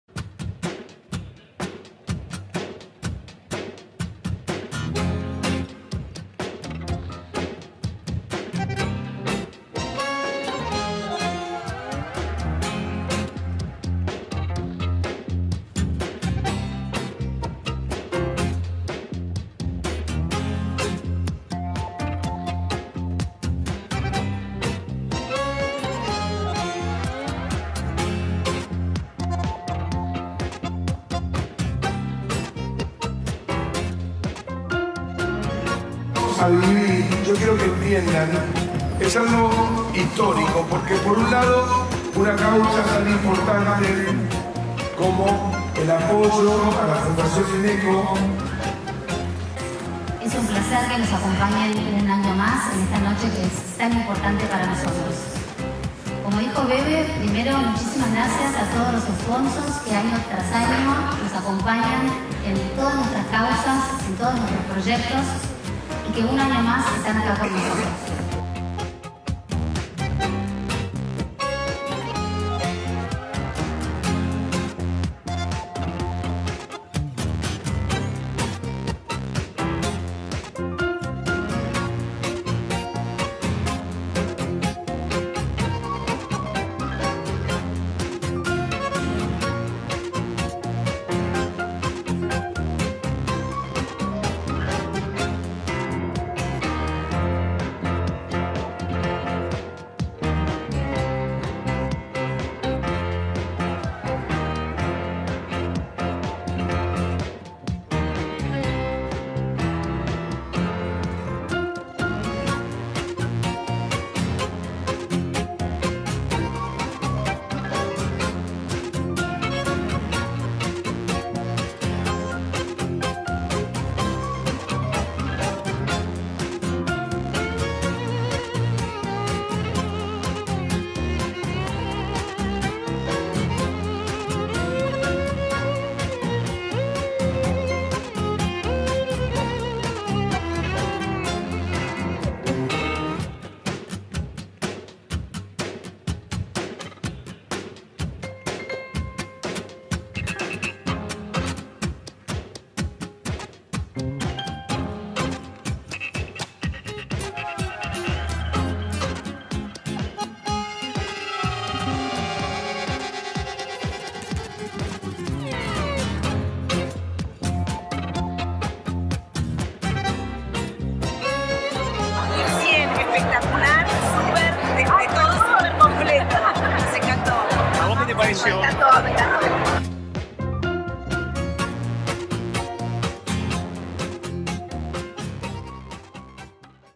3er. Concierto a beneficio de los programas de Salud Mental de Fundación INECO.
Este año, BAJOFONDO, liderada por Gustavo Santaolalla, en el 3er. Concierto a beneficio de los programas de Salud Mental de Fundación INECO.